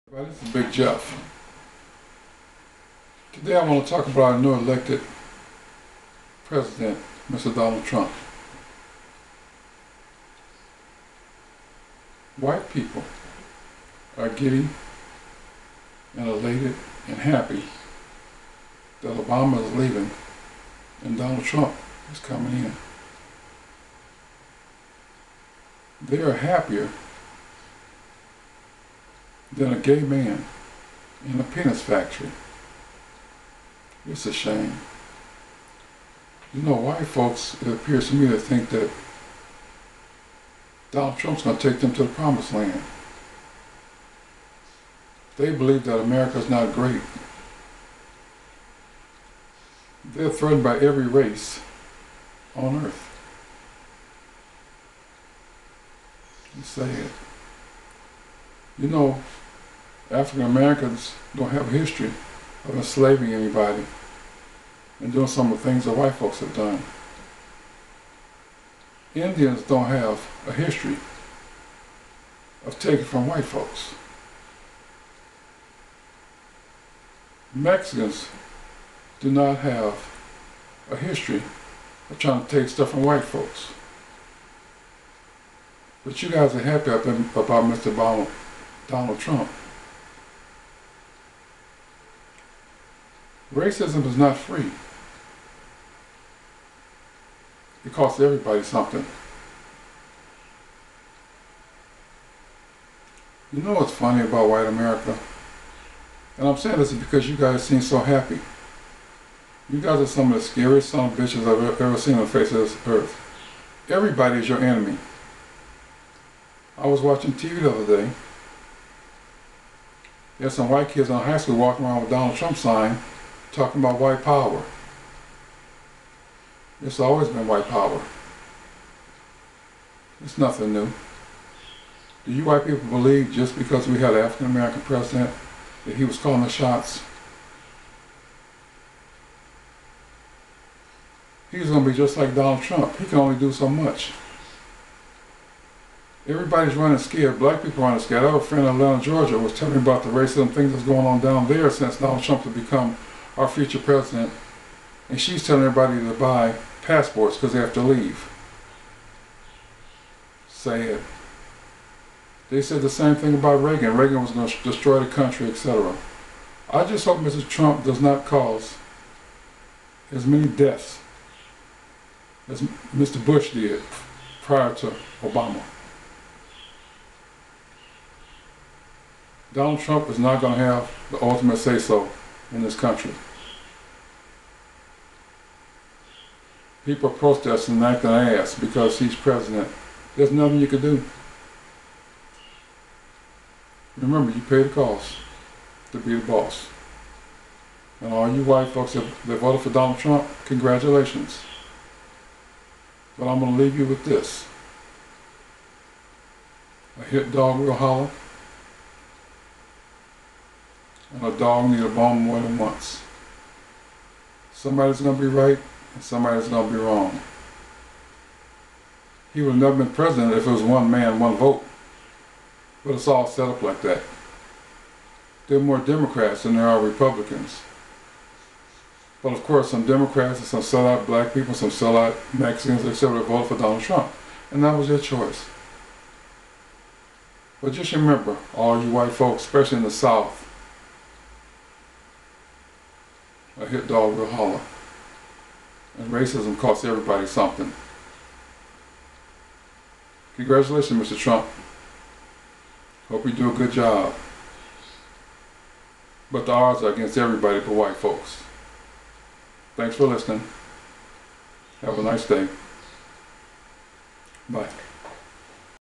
(timeless-answers.net) One Black Man's Opinion on American's new President Donald Trump.
(timeless-answers.net) This Video is about White American's New Great White Hope (Mr. Donald Trump) They believe he will make America Great Again. White American's are happier than a gay man in a penis factory. Racism isn't free...Racism cost everybody something. America NEED JUSES ASAP... Man cannot save himself.